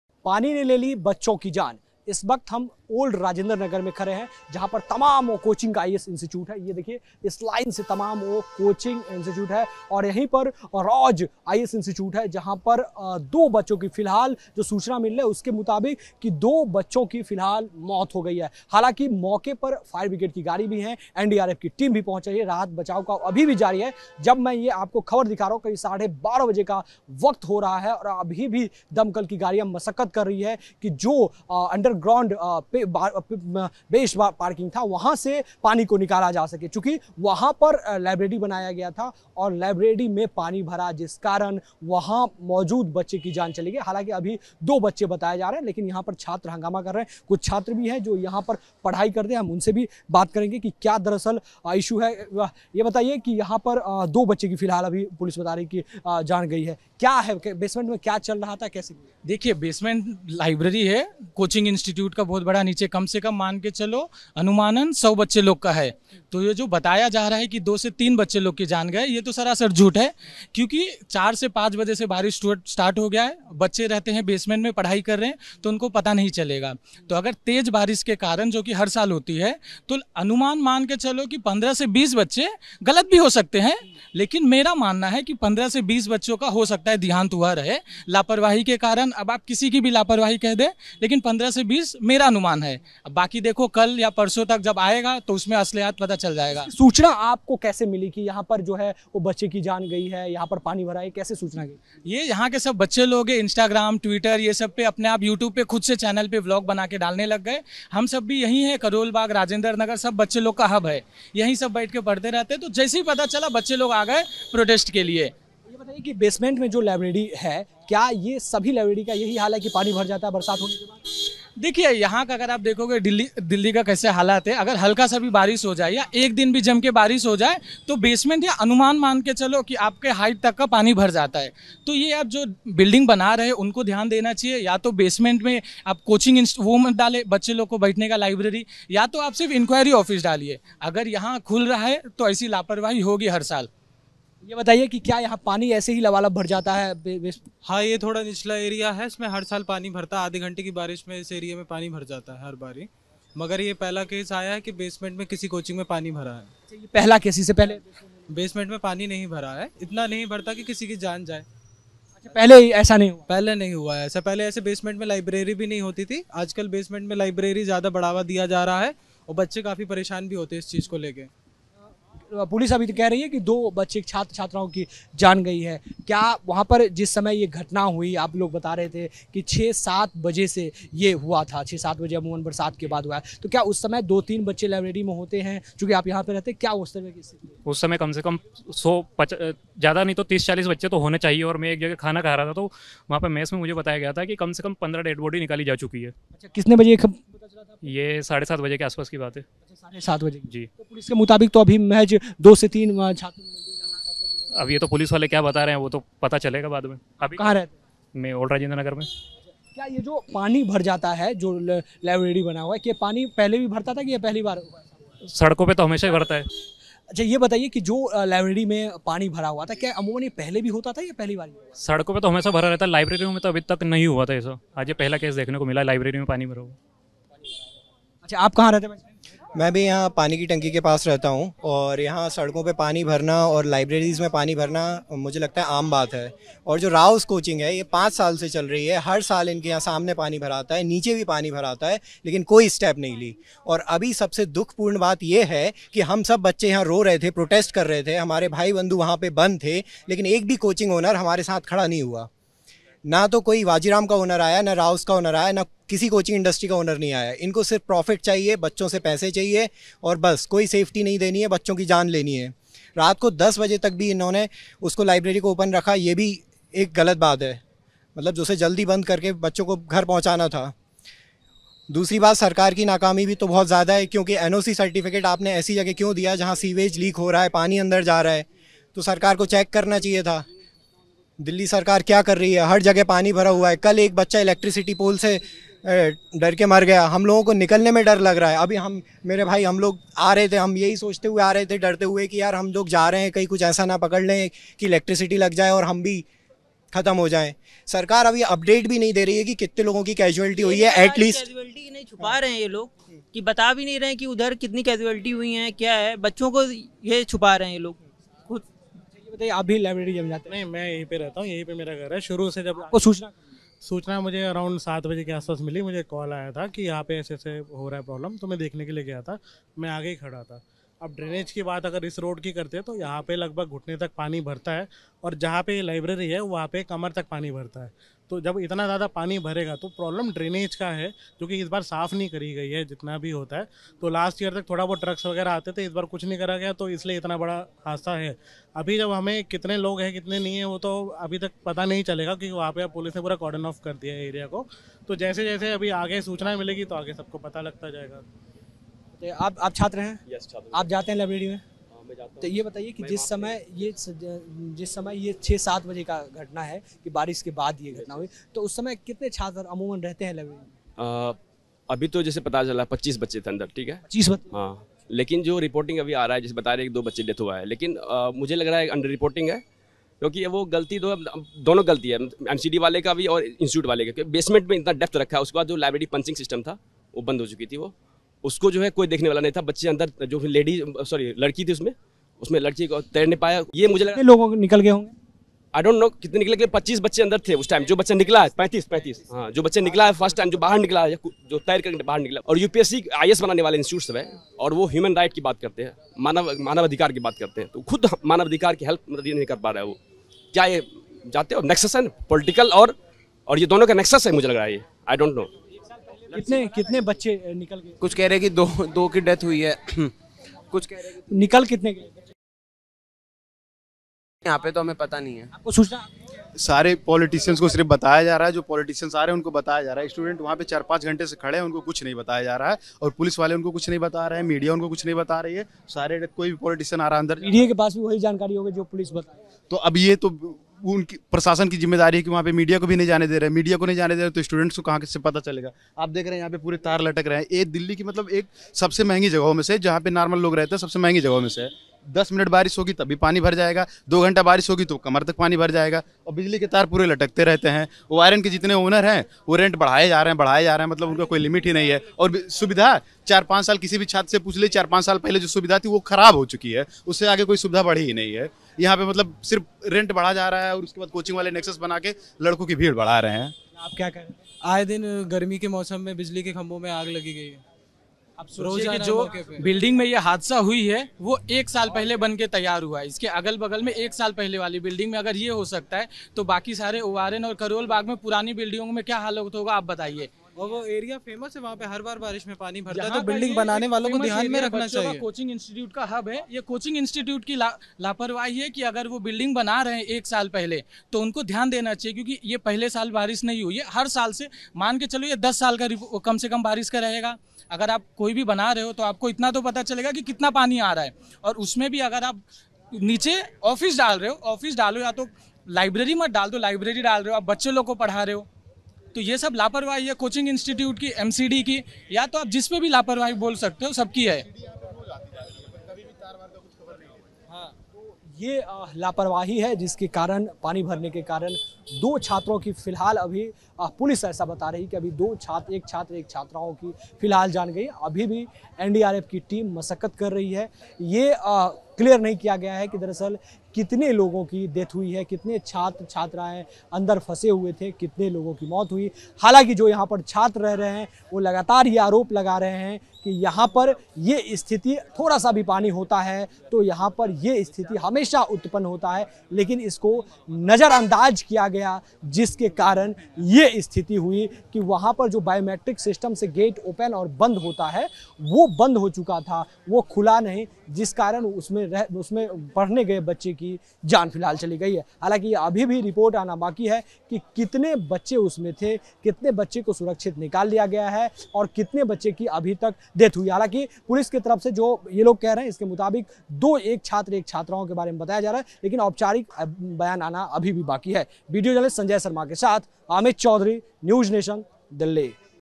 Delhi Coaching Incident : दिल्ली कोचिंग हादसे पर गुस्साए छात्रों ने क्या कहा ? Top News
News State Apps डाउनलोड करें:
Download Android and iOS Apps for more updates: